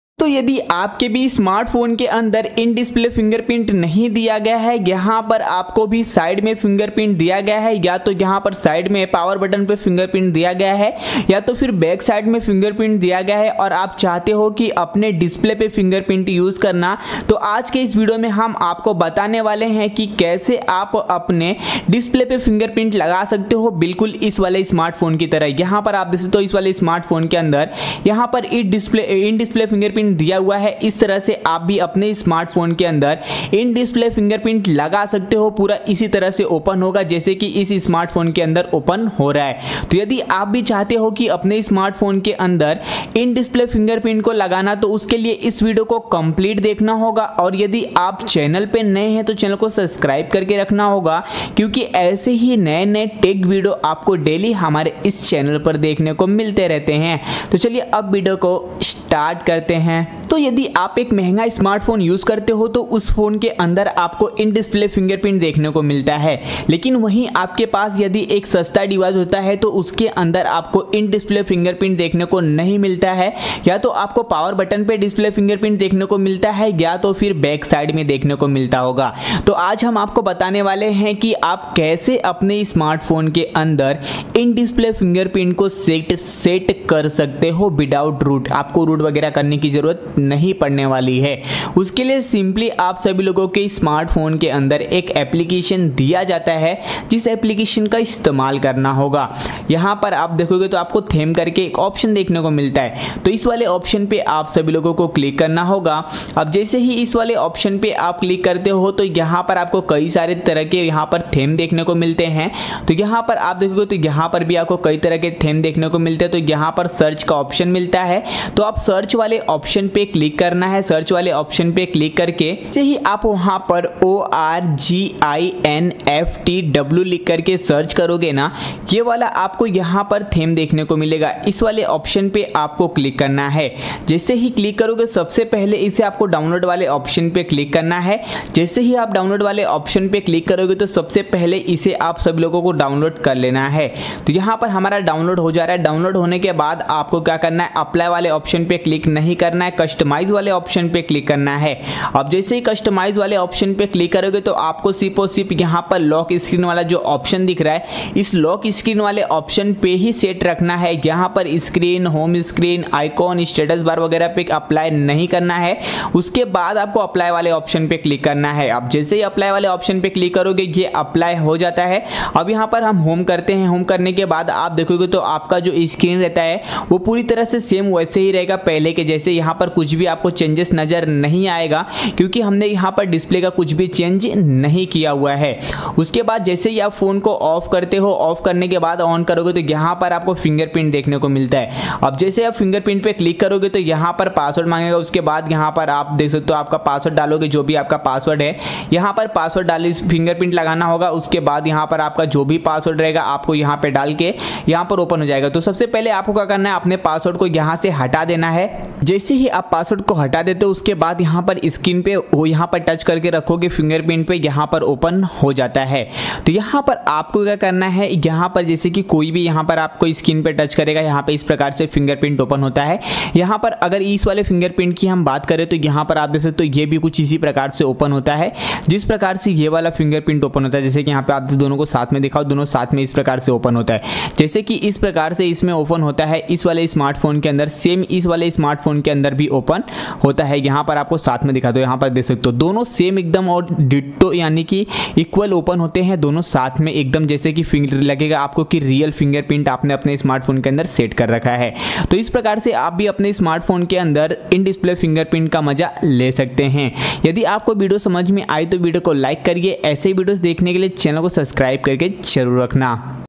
Kisi bhi Phone me Display Fingerprint Lock Lagao | Display Fingerprint Lock for any Android Phone
fingerprint

Hello 👋

Aj ke es video me mai apko batane wla hu ki kaise ap apne phone me real wla fingerprint lock ko laga sakte ho agar ap bhi apne Android phone me real wla fingerprint ko activate karna chahte ho to pura video end tak jarur dekhna bina kisi app ke madada se ap fingerprint apne phone me lga sakte ho.

Your Quaries ,